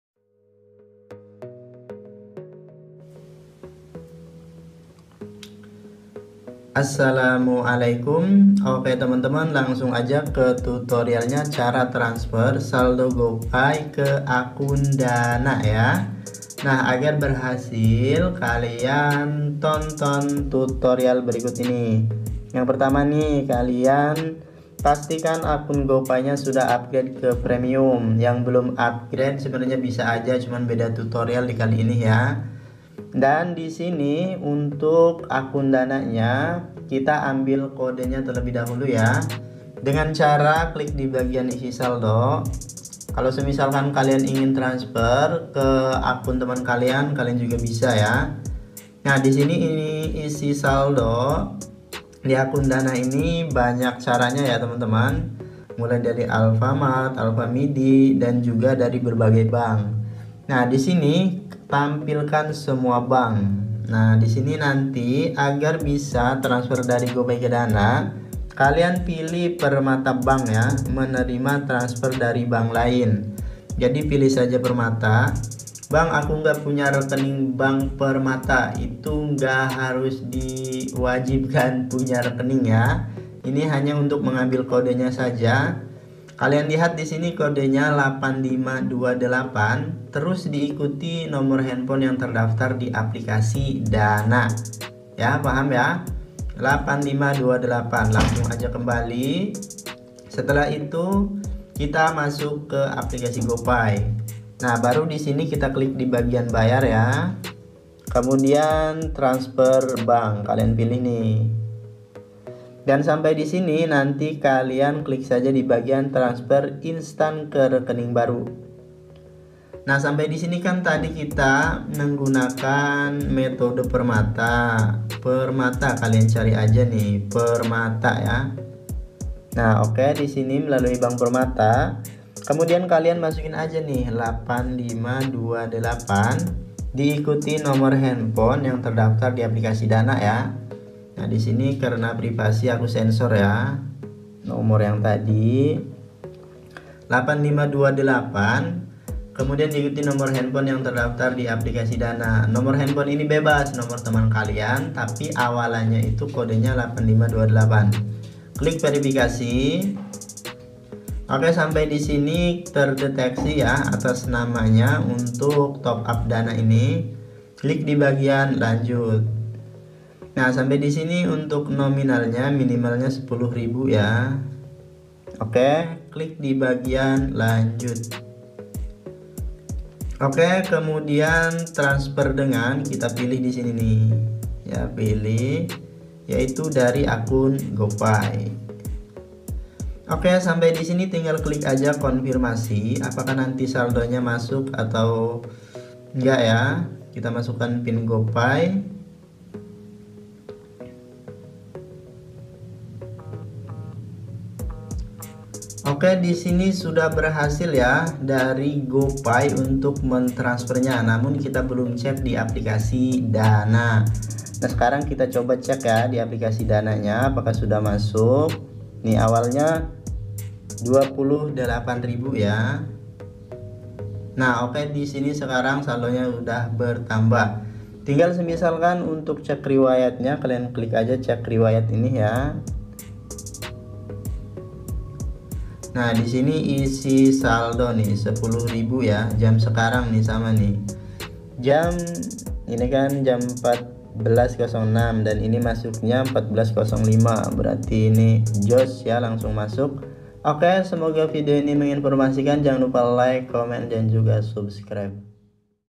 ✅️Cara isi Saldo Dana Lewat Gopay - Top Up Dana Lewat Gopay
assalamualaiku. ok teman2 langsung aja ke tutorial Cara isi Saldo Dana Lewat Gopay - Top Up Dana Lewat Gopay